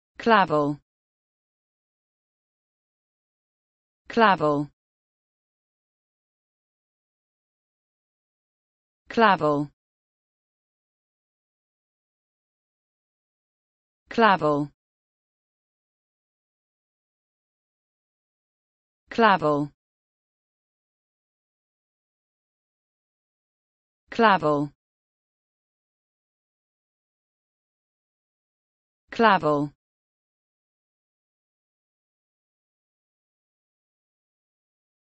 the correct pronunciation of clavel in English.
This video shows you how to pronunciation clavel in English.